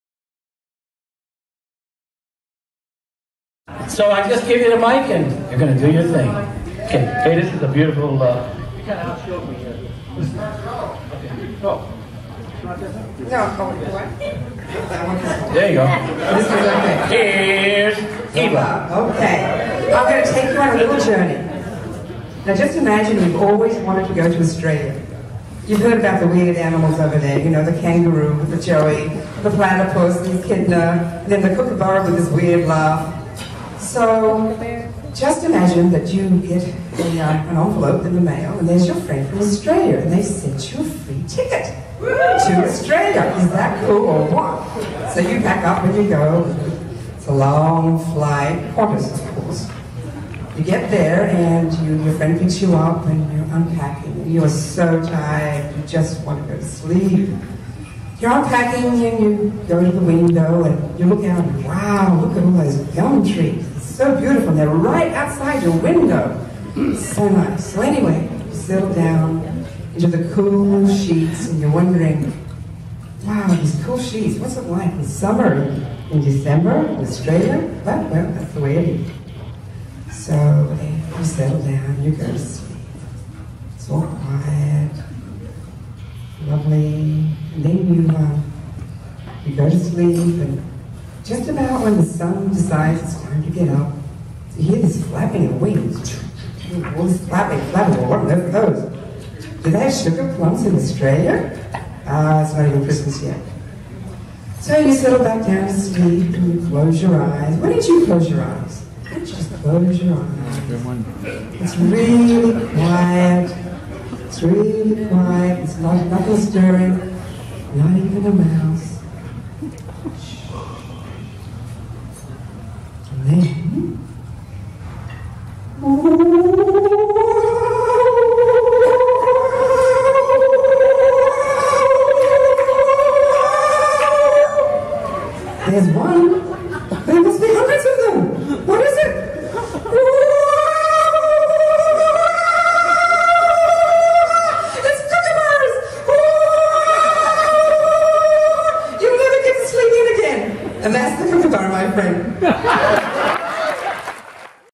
Some Aussie fun at Vineyard at The River's Christmas Dinner - 2013
This video is about a little bit of Aussie fun at the 2013 Vineyard at The River's Christmas Dinner.